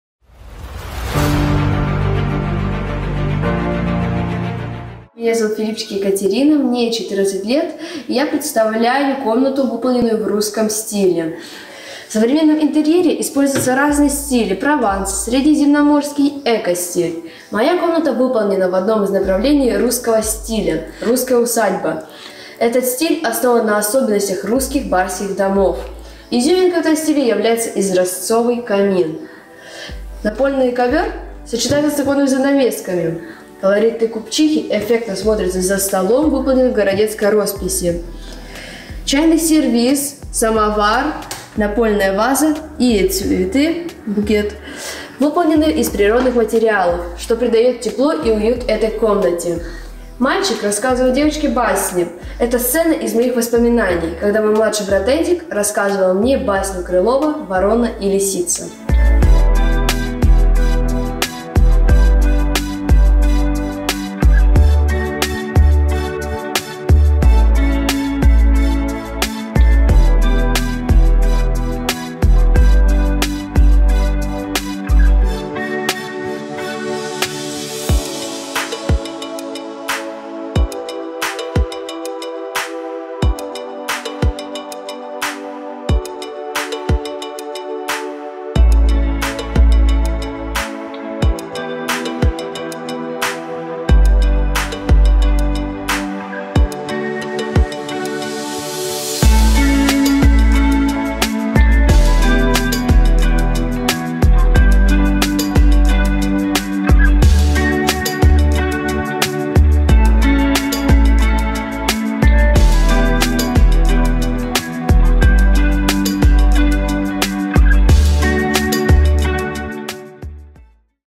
Golden Time Talent | 26 Season | Filippchik Ekaterina | GT26-6636-7379
Имя участника или коллектив: Filippchik Ekaterina

Страна: Russia

Город: Большая Мартыновка

Название конкурсной работы: Дизайн комнаты в русском стиле.

Размеры работы: 50,40,50

Материал исполнения: Фанера,картон,глина,ткань,природные материалы

Технология изготовления: Лепка,плетение,художественная роспись

Педагог: Жукова Марина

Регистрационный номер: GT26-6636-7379

Номинация: Дизайн

26 Season

Участие в других конкурсах и фестивалях / творческие достижения: Екатерина неоднократный обладатель Гран-При и Лауреат 1 степени в очных международных и всероссийских конкурсах в Ростове -на-Дону,Саратове,Санкт-Петербурге,Сальске.,Волгограде.